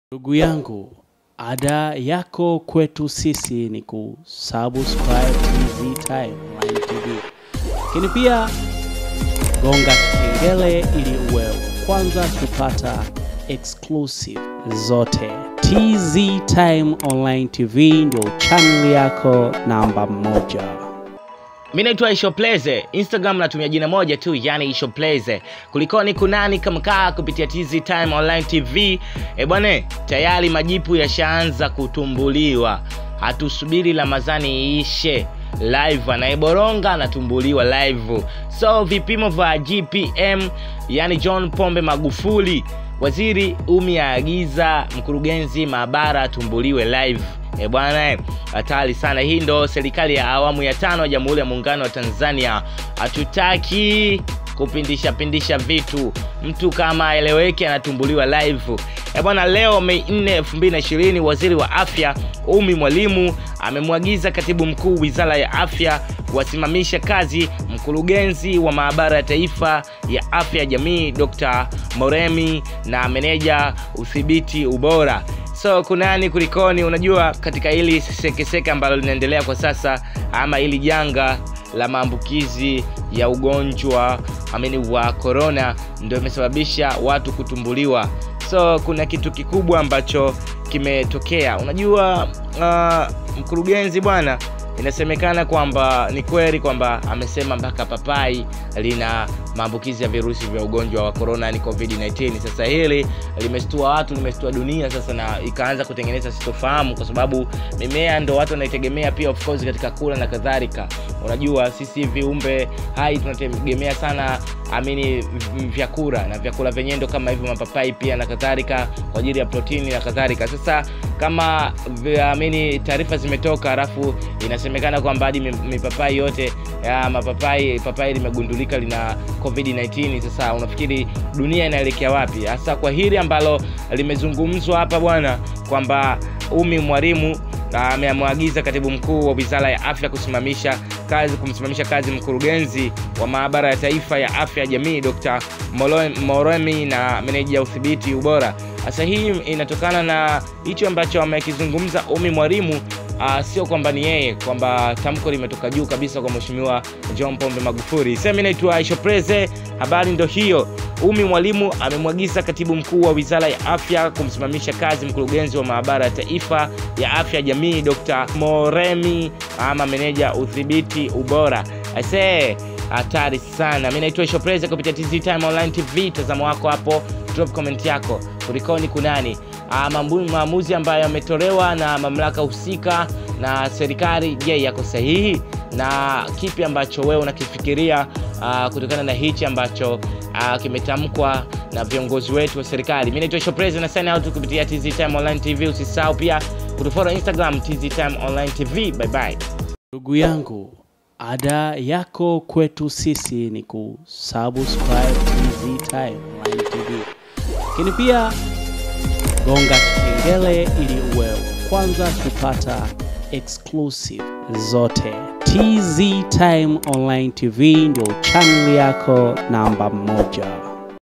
.MAGUFULI AAGIZA MKURUGENZI WA WIZARA YA AFYA KUFUKUZWA KUTOKANA NA UBATILI WA VIPIMO VYA CORONA
tztimeonlinetv (magufuli amfukuza waziri wa afyaaaa)